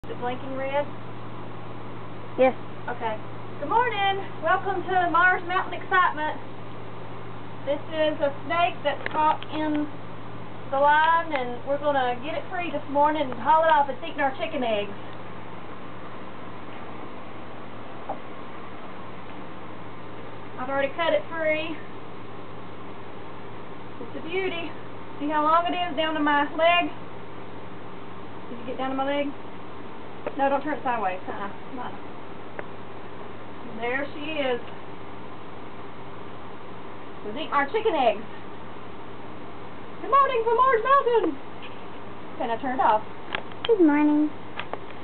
Black snake caught in chicken coop #2
The snake was being cut free in the first, short video...missed that video part with the camera...see part #3 for release of snake near our local waterfall down the road.  Black snakes are are friends!